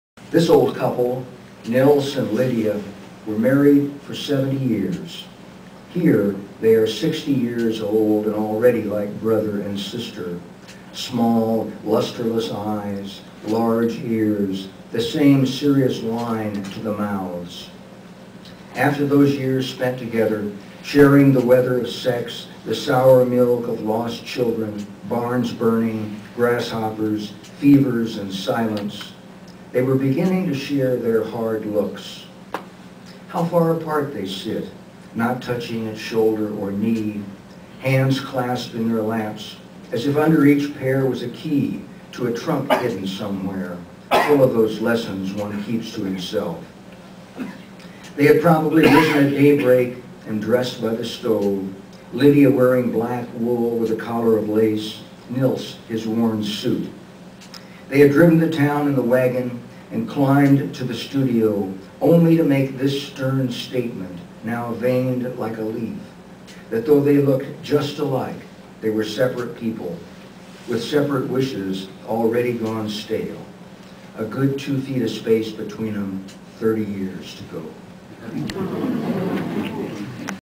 An old photograph, read by Ted Kooser
Ted Kooser presenting to Round Up at York College, May 23, 2008